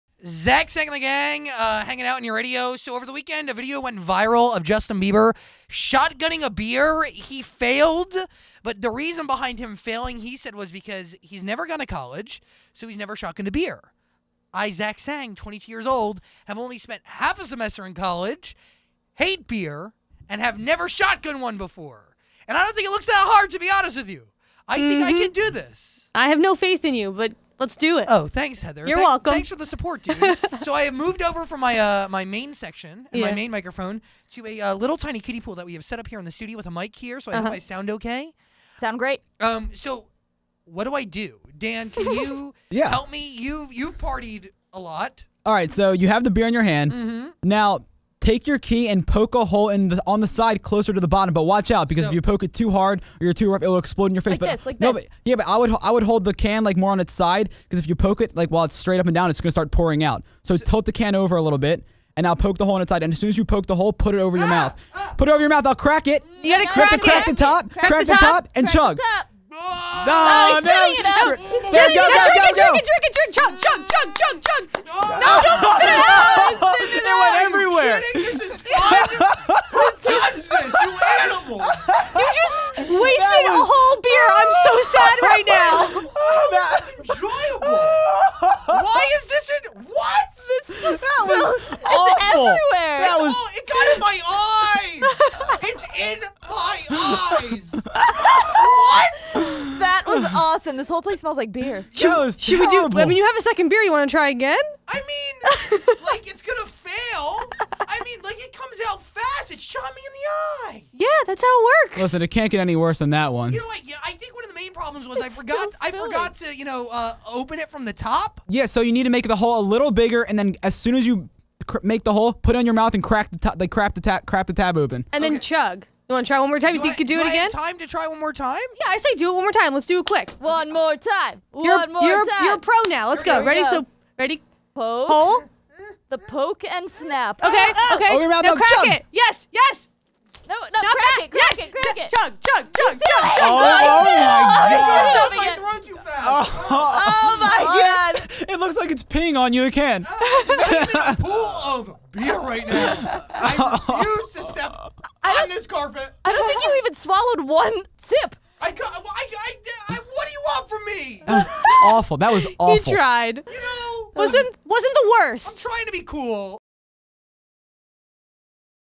Zach Attempts to Shotgun a Beer
Zach tried to shotgun his first beer ever...
He failed miserably.